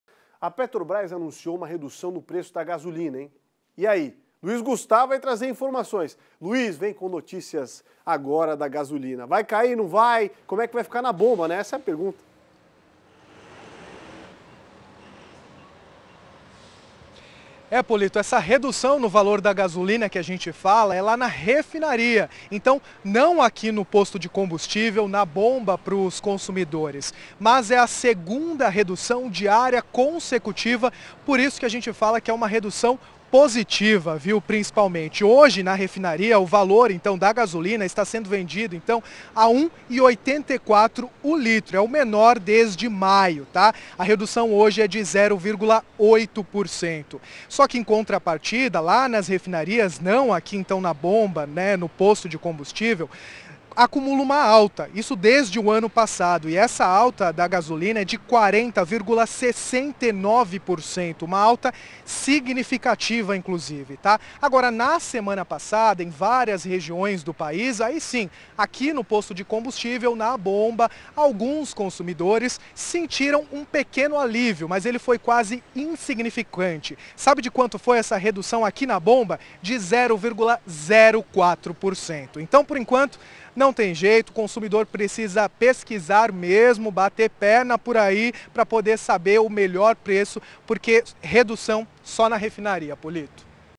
Preço da gasolina vai ter nova redução
Valor do combustível é o menor desde o mês de maio. Na bomba alguns consumidores sentiram um pequeno alívio.